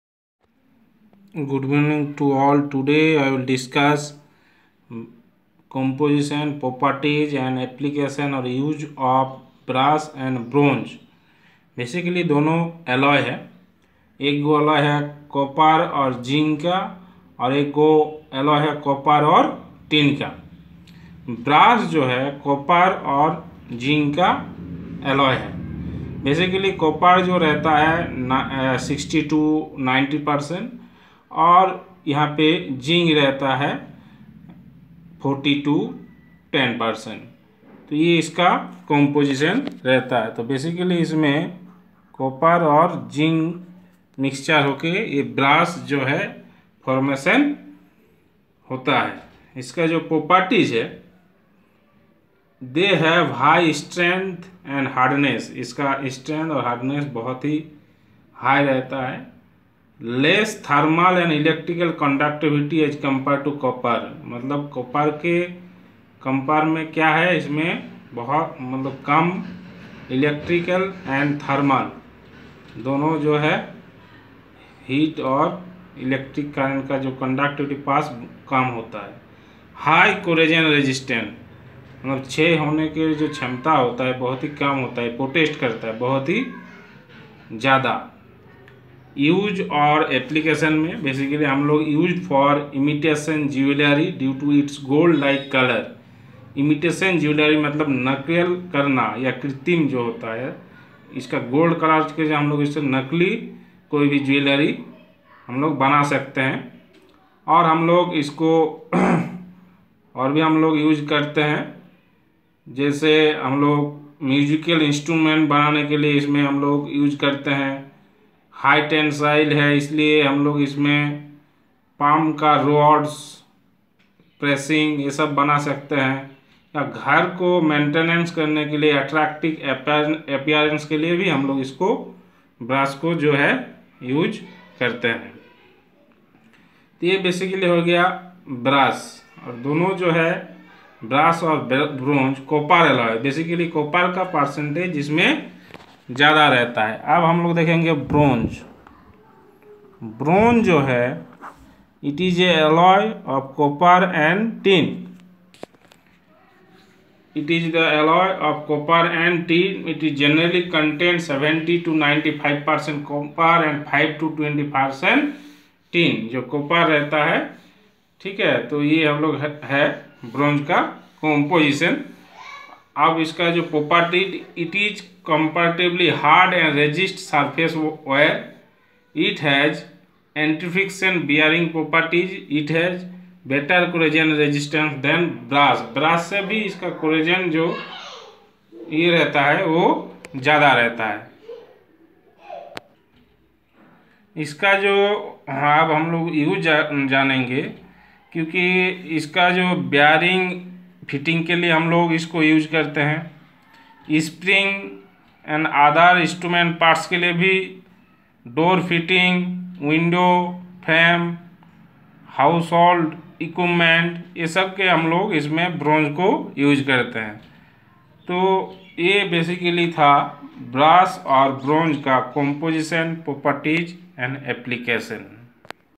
Metals and Alloys ( lecture -6)
Composition, Properties and Application  of Brass and Bronze